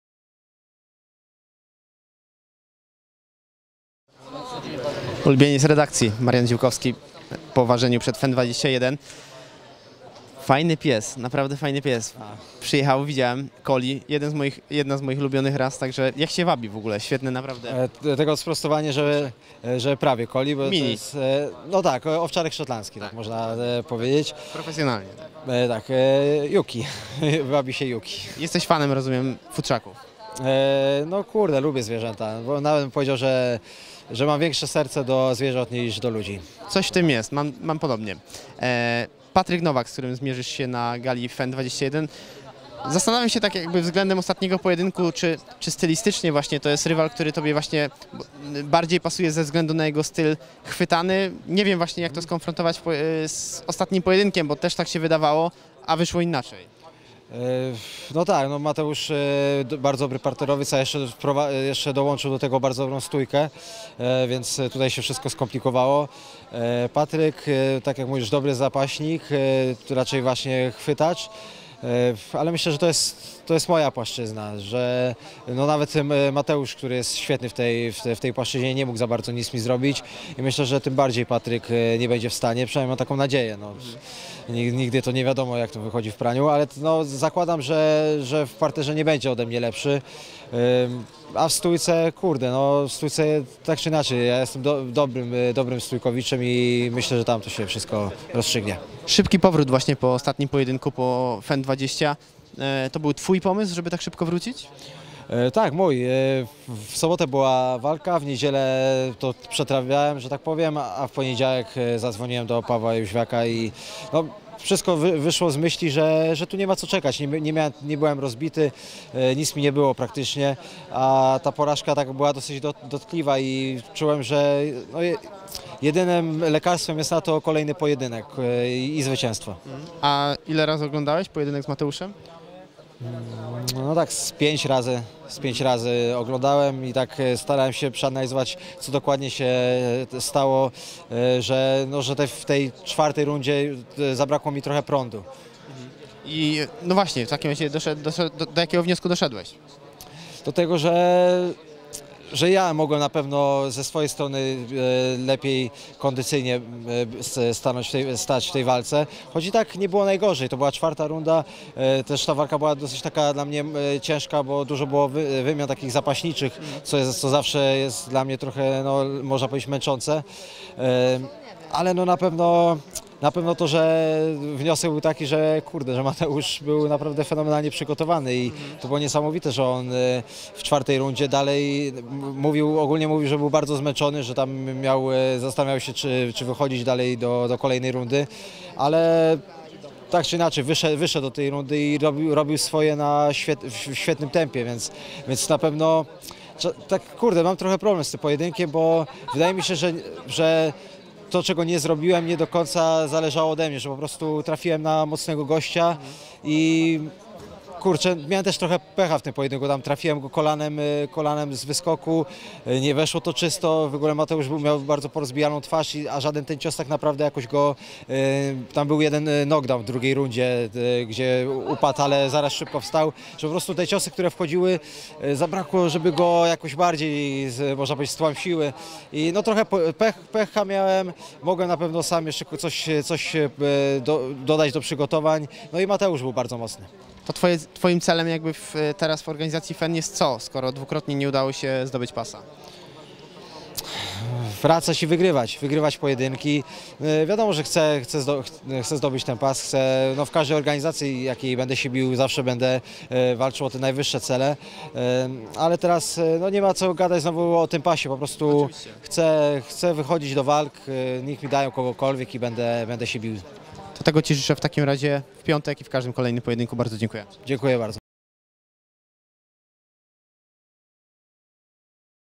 FEN 21: Marian Ziółkowski nie widzi zagrożenia ze strony Patryka Nowaka
Marian Ziółkowski obszernie o zbliżającym się wyzwaniu z Patrykiem Nowakiem na FEN 21, gdzie powiedział nam dlaczego nie obawia się o rezultat pojedynku.